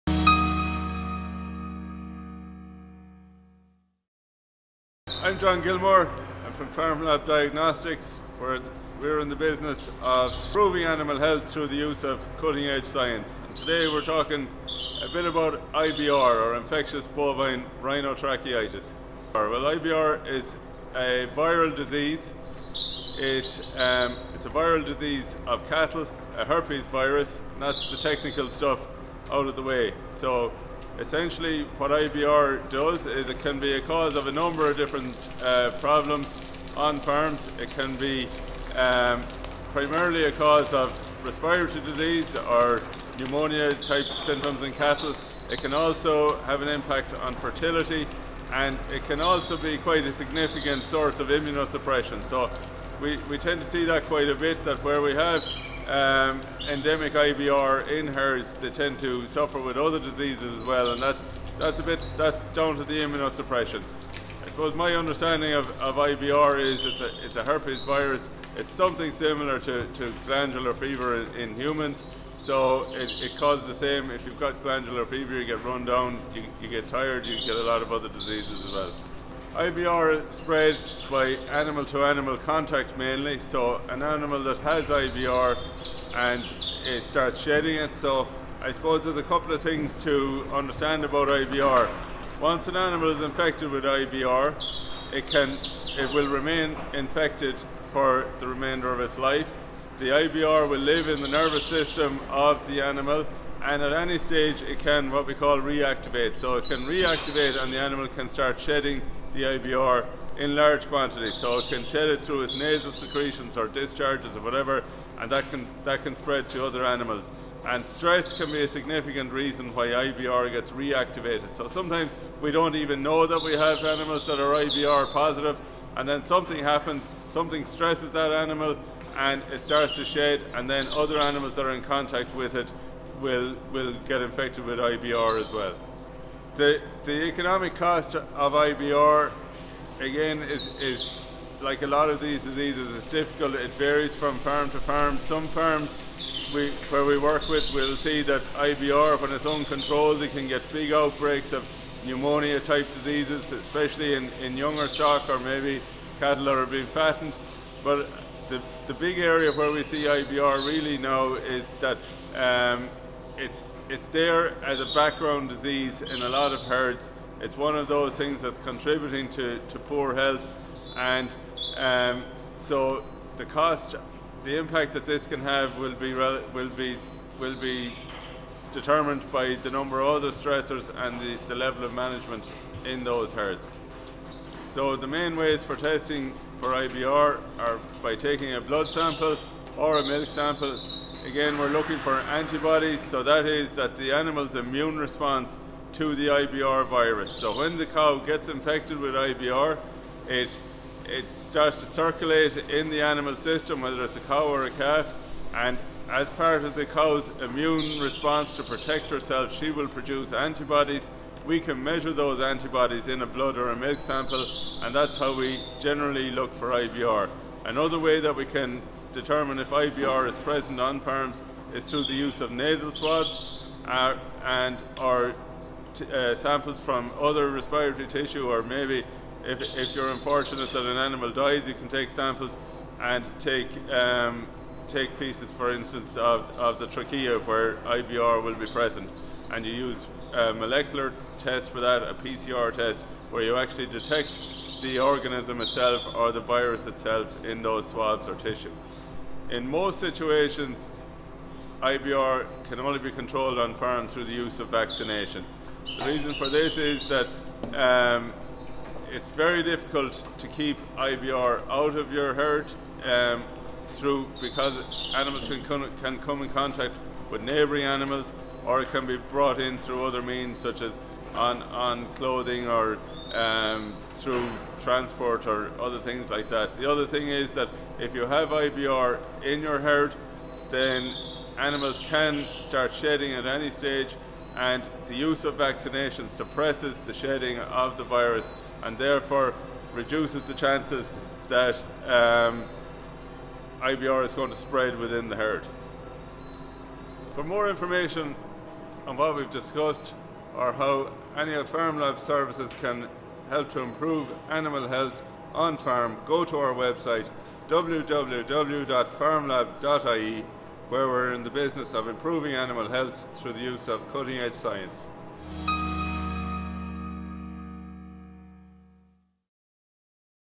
IBR Explained by Vet John Gilmore
Farmlab Diagnostics  Owner and Vet  John Gilmore talks through Infectious Bovine Rhinotracheitis.
He explains the symptoms and the control options around the disease.
He also talks about the ways we can test for IBR here at Farmlab Diagnostics.